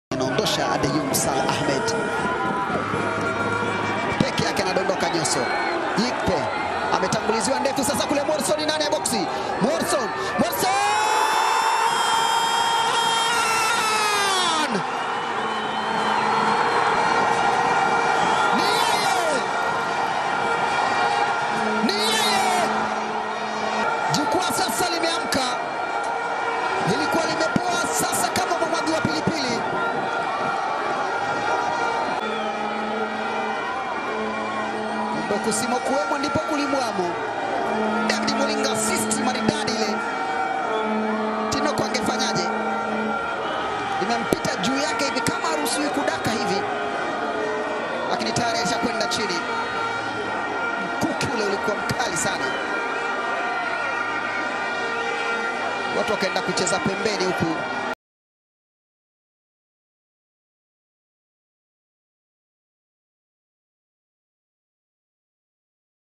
TAZAMA GOLI LA BERNARD MORRISON/ YANGA 1 - 0 KAGERA SUGAR
Goli pekee la Yanga ukiwa ni mkuki mwingine wenye sumu kali kutoka kwa Bernard Morrison, safari hii akiuelekeza kwa Kagera Sugar. FT: Kagera Sugar 0-1 Yanga.